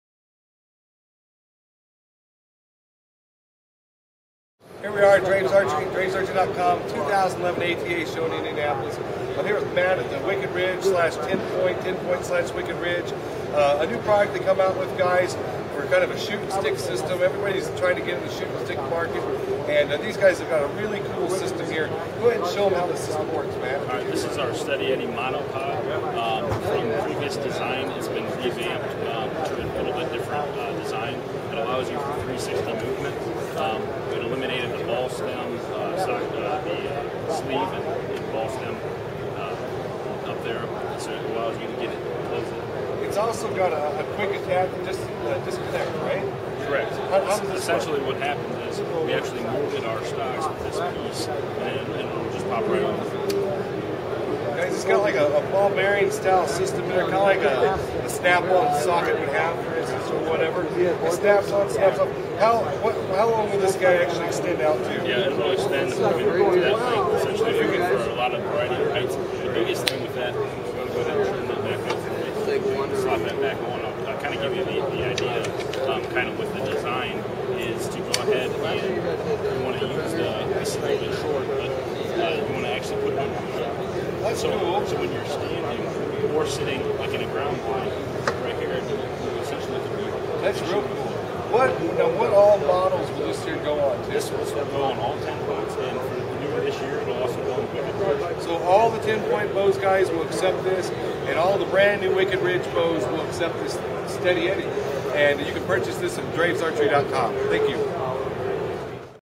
2011 TenPoint Steddy Eddy
Check out some of the products we came across while at the 2011 ATA Trade Show in Indianapolis.